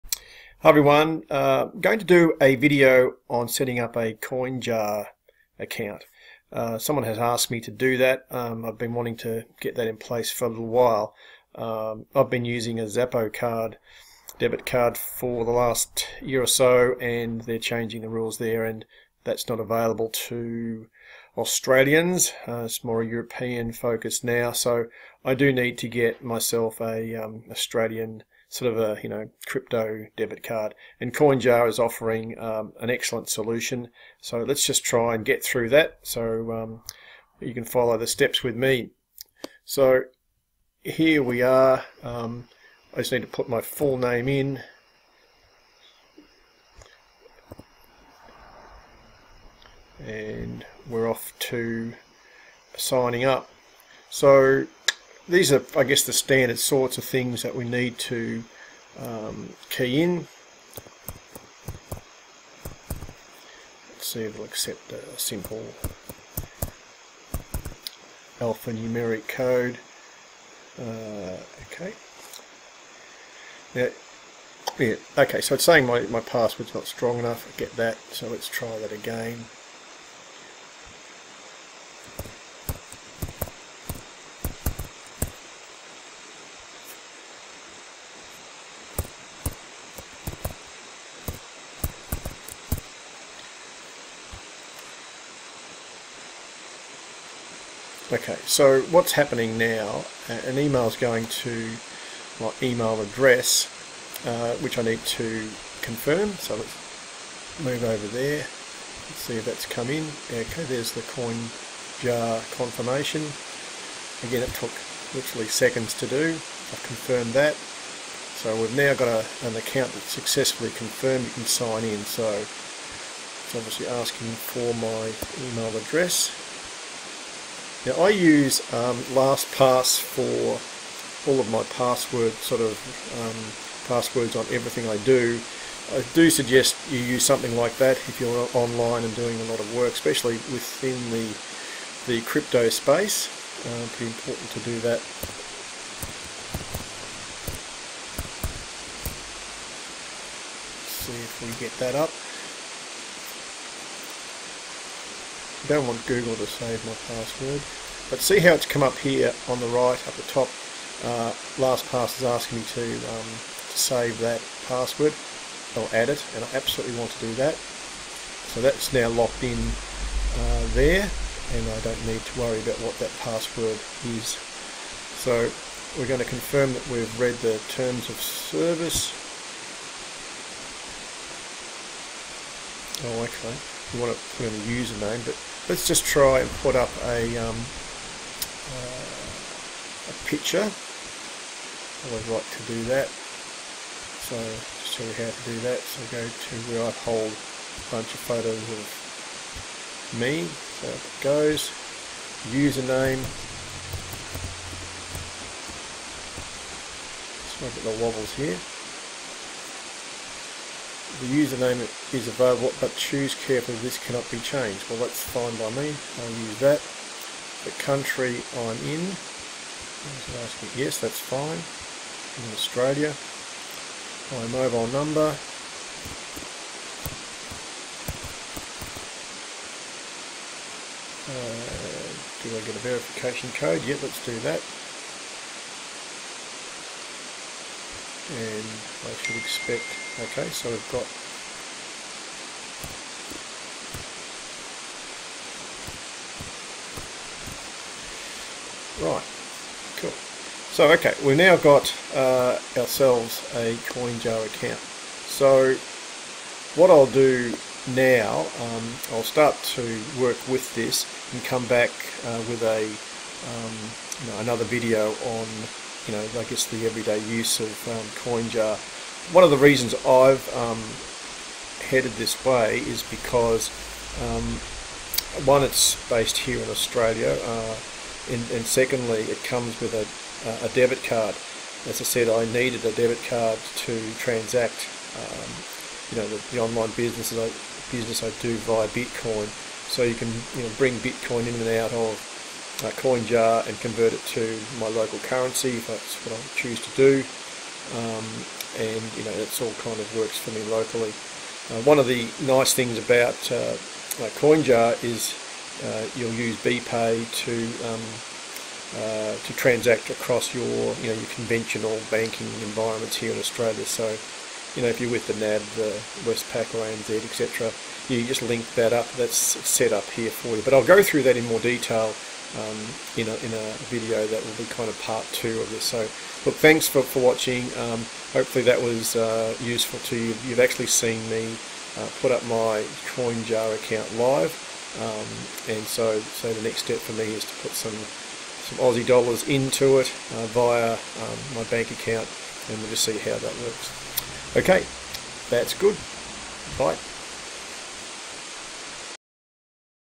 How To Create A Coinjar Account - Part 1
Step by step over the shoulder video that shows you how to set up a Coinjar Account.

This is an EASY and safe way to buy/sell and exchange Bitcoins and send to and from your Australian bank account.

In this video we go through the steps of creating your account. The Part 2 video we will be verifying my, adding my bank account and how to buy Bitcoins using Coin Jar from your Australian Bank account.

I will also touch on sending and receiving Bitcoins using the same Coin Jar account

Follow the video, I try and cover everything... but go back and watch it a few times might be best to get setup and owning bitcoins in no time!